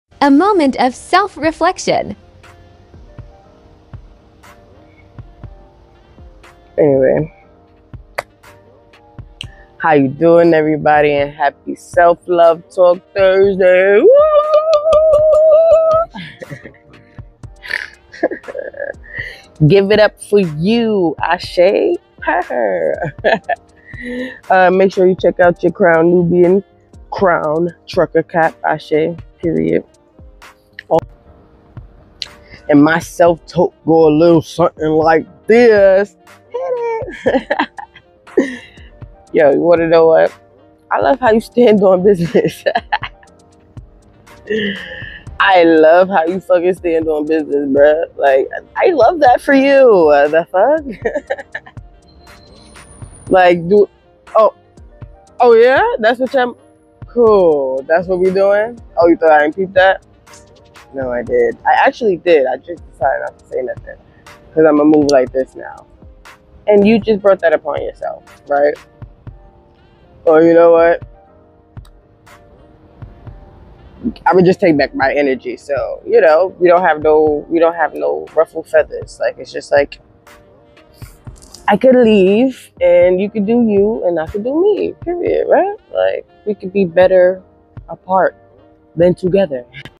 Happy Self-love Talk Thursday 👄 🌹🫶🏾✨️
Happy Self-love Talk  Thursday 👄... No I'm not dolled up 🤷🏾‍♀️ I love myself in my rawest form ... if you are offended.. please work on loving on self in your purest for. It's not easy but very much needed... have a great Thursday Loves